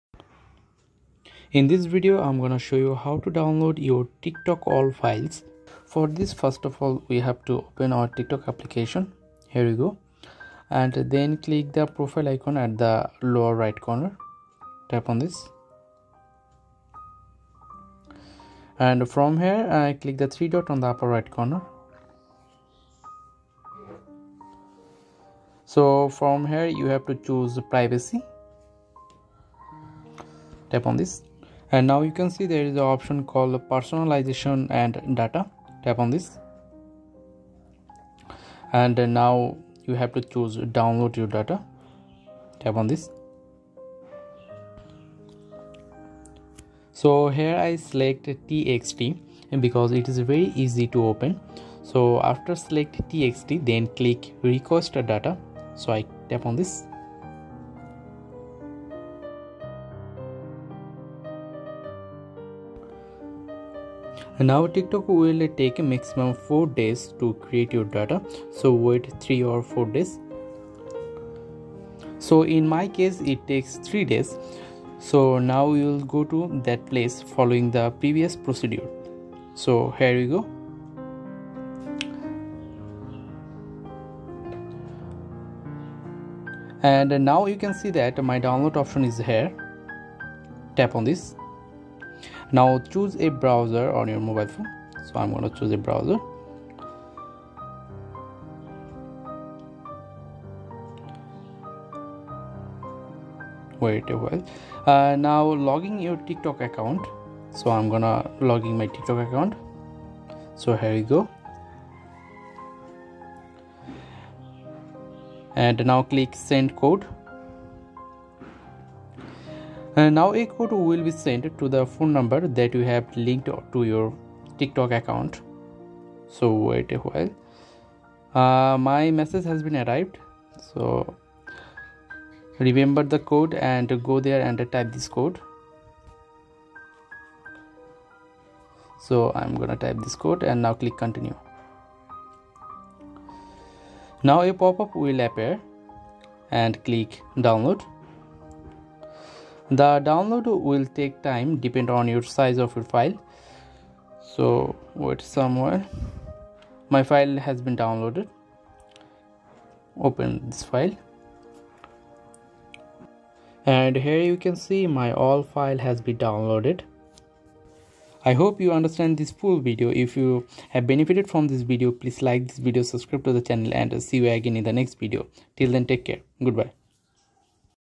How to Download TikTok All Data include All Videos 2021
Today we will learn How to Download TikTok All Data include All Videos

Video Step:
First of all open your Tiktok application then go your profile and click the three dot on the upper right corner then tap on privacy and find out Personalization and data and the rest of the process is in the video.

Copyrights: Tech Process